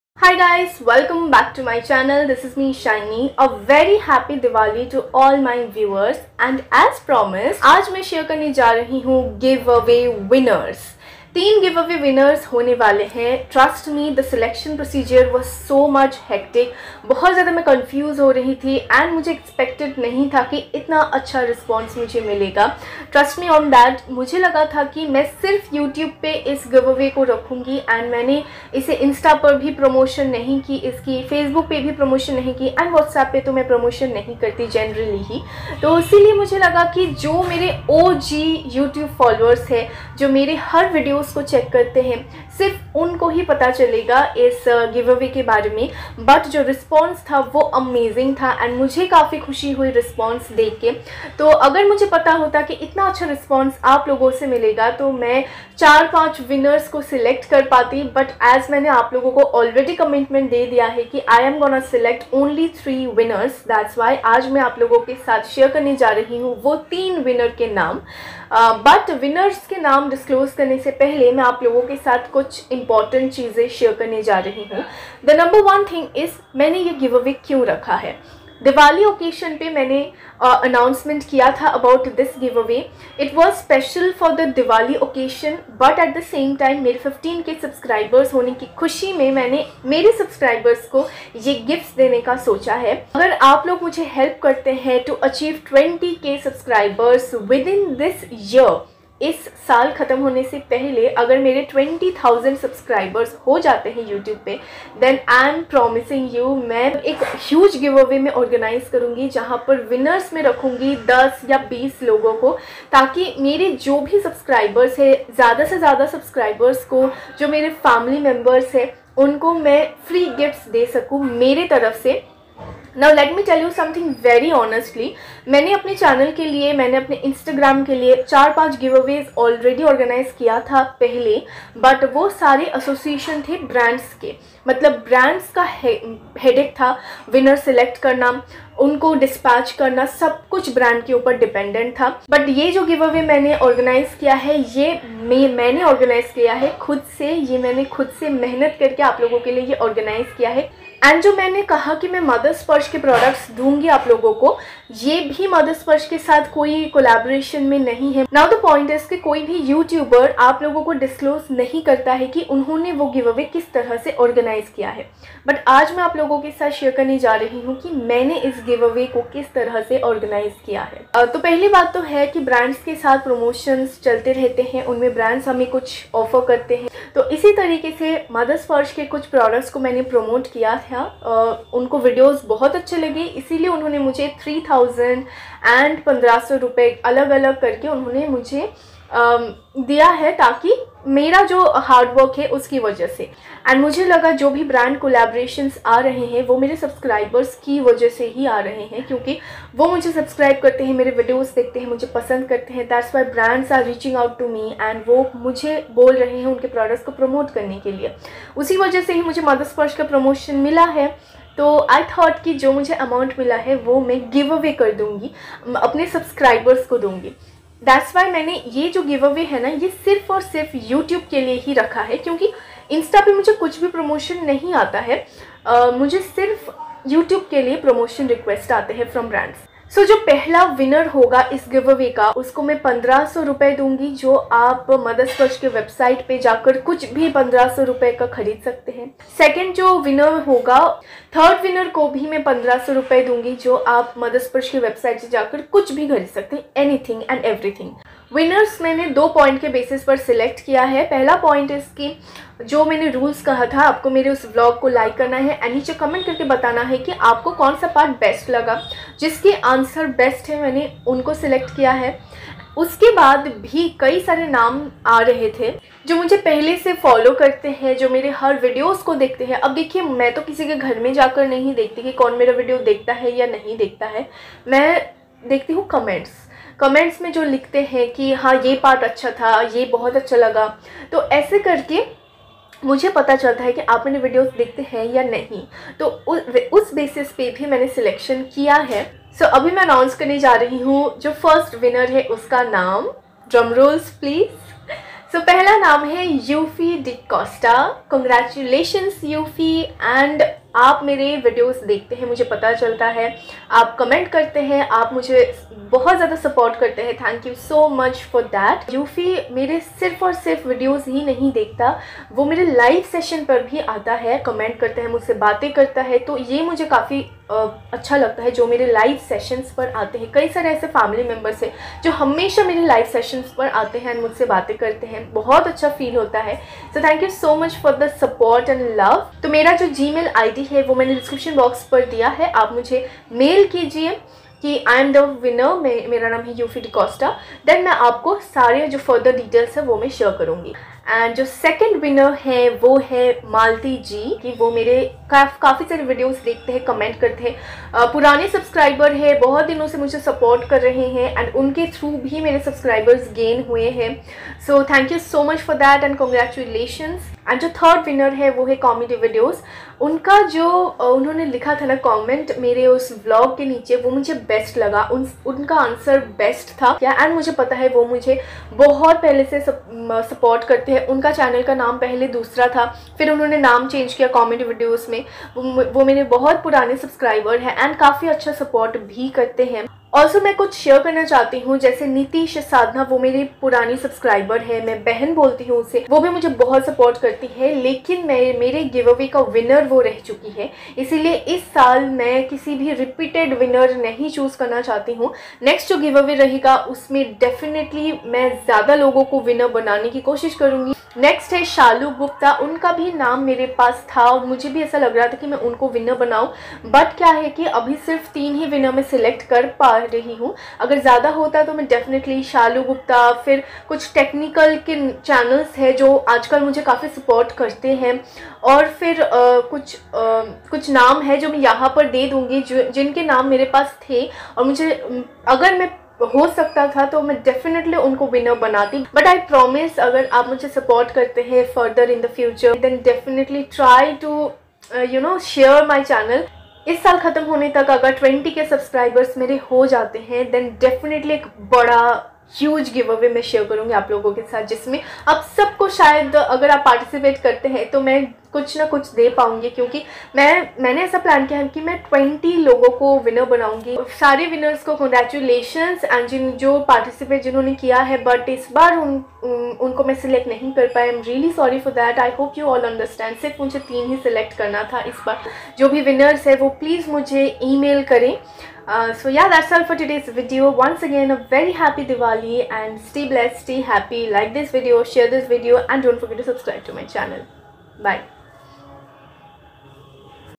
*GIVEAWAY* Winners Announcement / How I organized this giveaway? Brand Payments
Congratulations to all 3 winners.
Please Support me like this only.